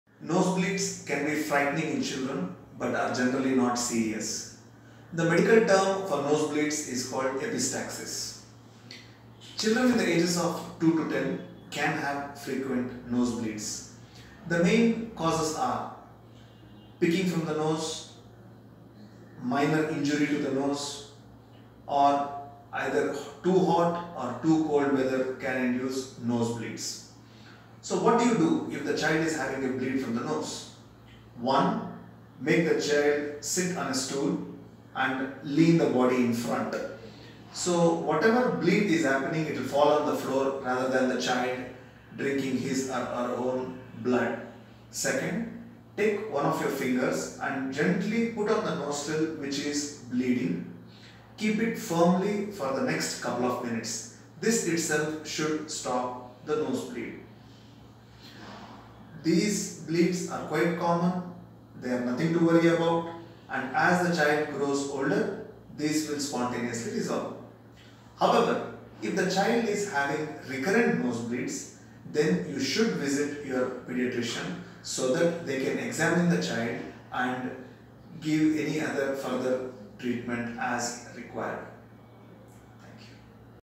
Nosebleed/Epistaxis in Children - Dr Adarsh Somashekar
Paediatrician/Co-founder, Chairman & Director of Ovum Hospitals. A avid fitness enthusiast.